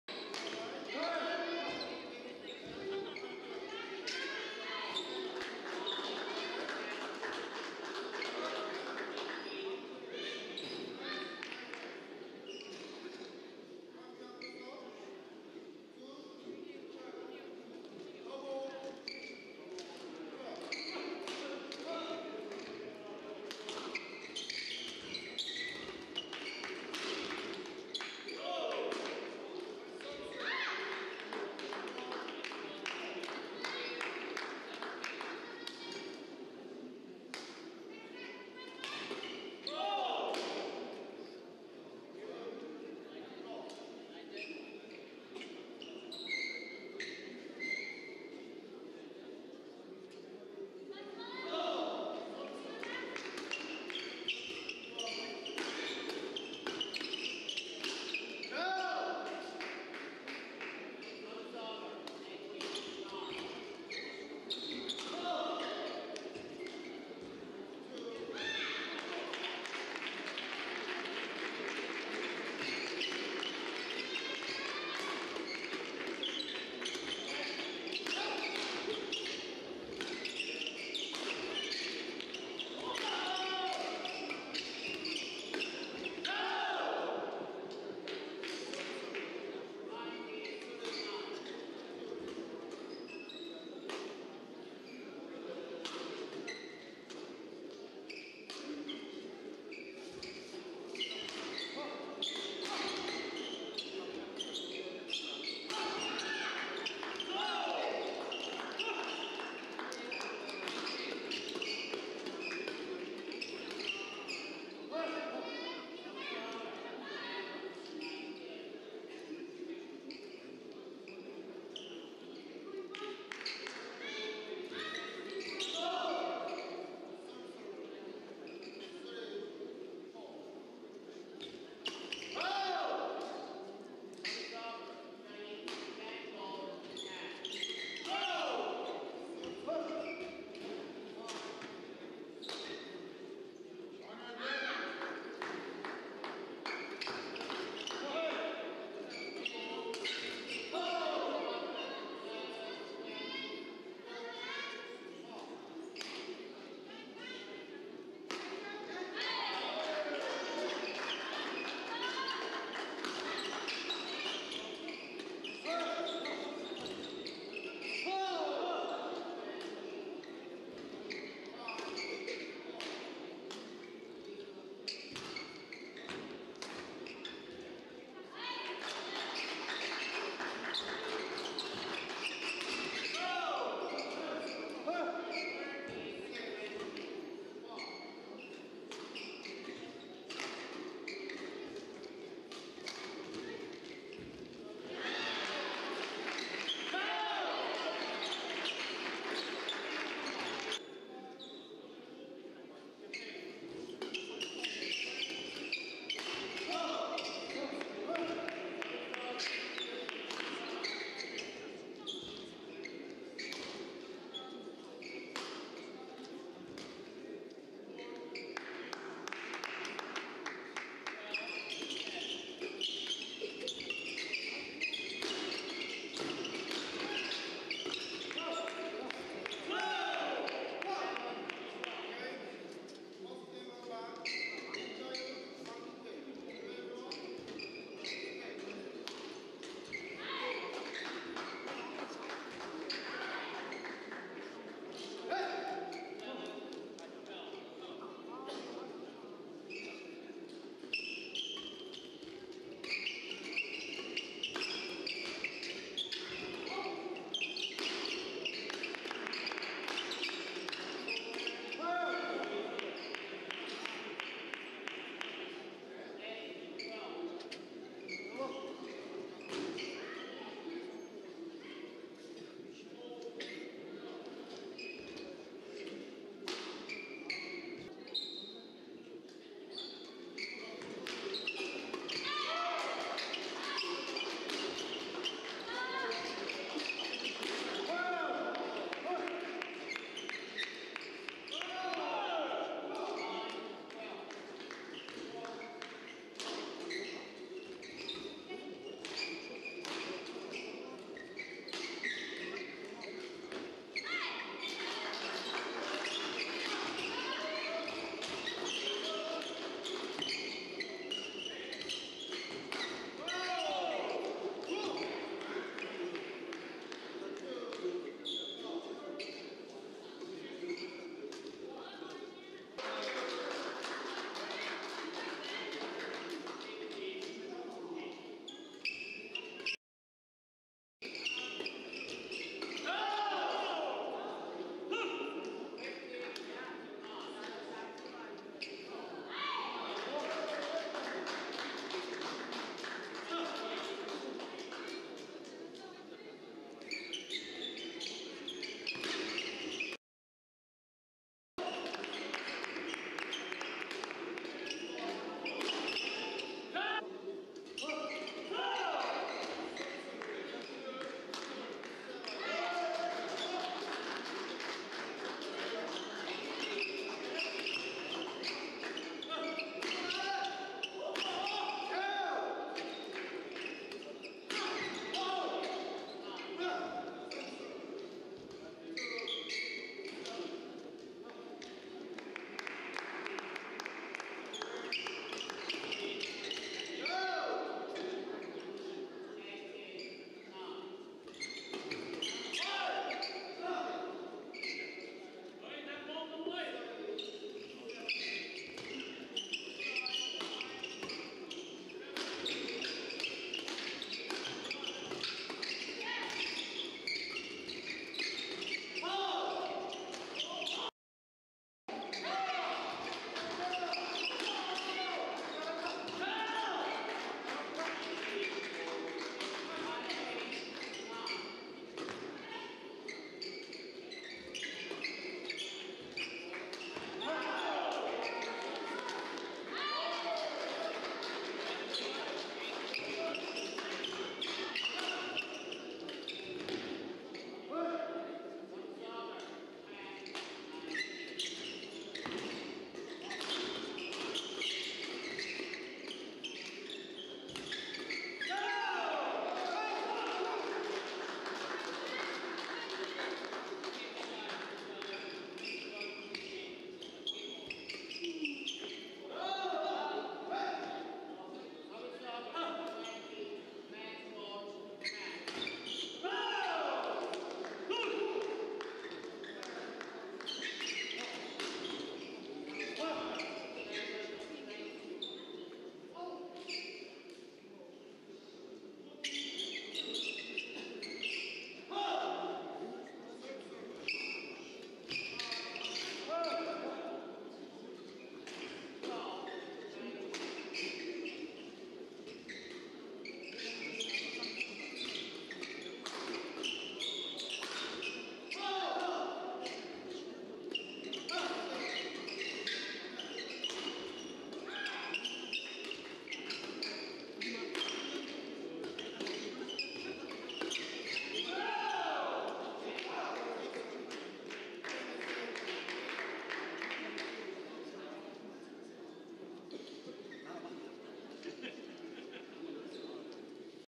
Korea Master 2016, R16 MD, Lee Jhe Huei/Lee Yang vs Kim Hui Tae/Lim Ji Soo
Korea Master 2016, R16 MD, Lee Jhe Huei/Lee Yang (China Taipei) vs Kim Hui Tae/Lim Ji Soo (Korea)

JEJU VICTOR 2016 Korea Masters Championships
BWF - Badminton Korea Association
Venue: Seogwipo Olympic Stadium
Address:  97 Jungsanganseo-ro Seogwipo-si Jeju, Korea